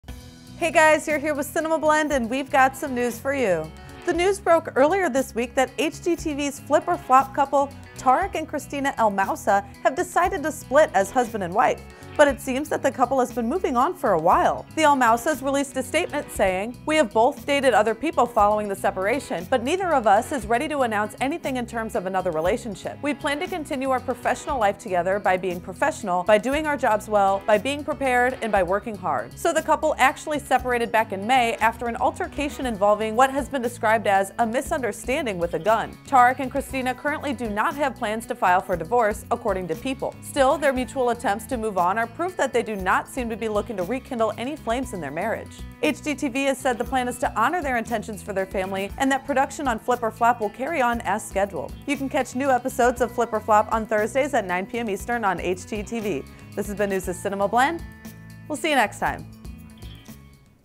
Seems Like The Flip Or Flop Couple Have Moved On In Their Personal Lives
Upcoming Movies & Movie Previews: Upcoming Movies in the Blend Film Database | CINEMABLEND